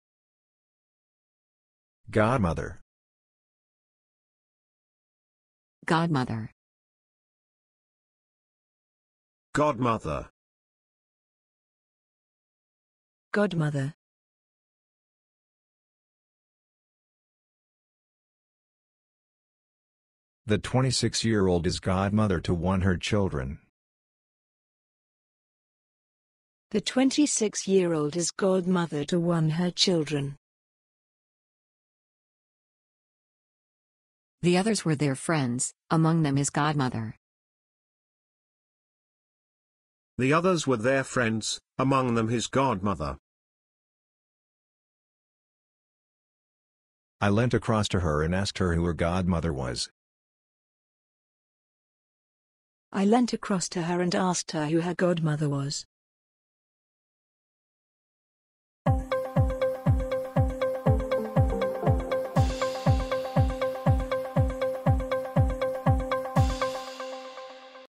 GODMOTHER pronunciation | Improve your language with bab.la
Improve your spoken English by listening to GODMOTHER pronounced by different speakers – and in example sentences too. Learn and love languages with bab.la.